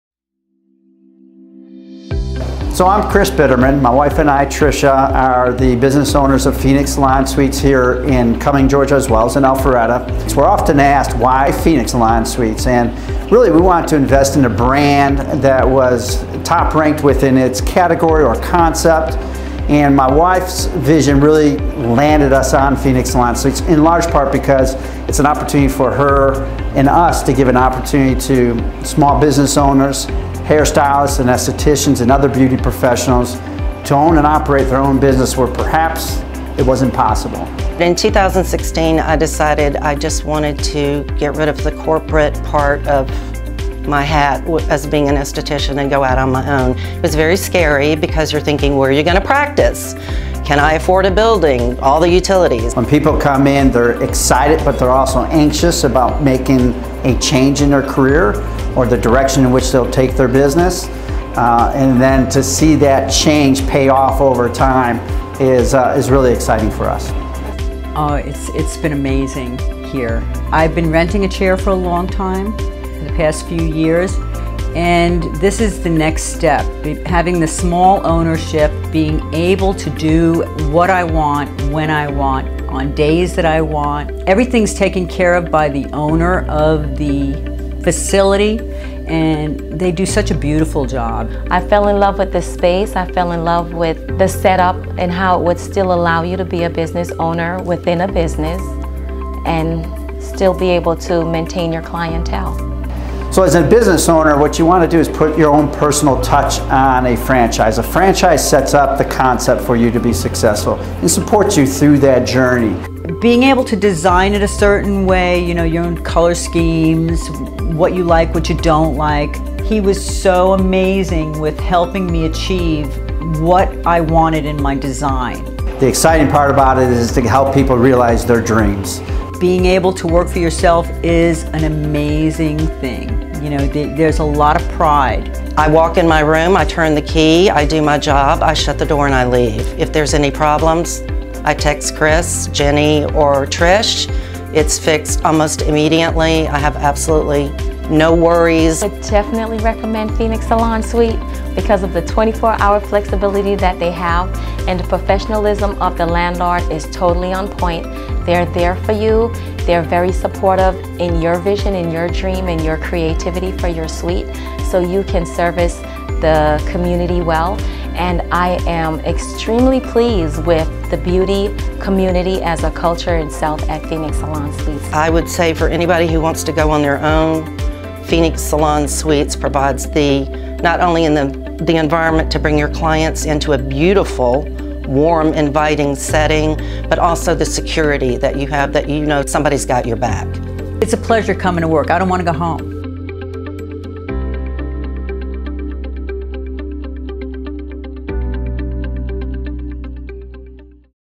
Phenix Salon Suites Alpharetta & Cumming Georgia
Phenix Salon Suites Alpharetta & Cumming offer you the opportunity to start your own salon business!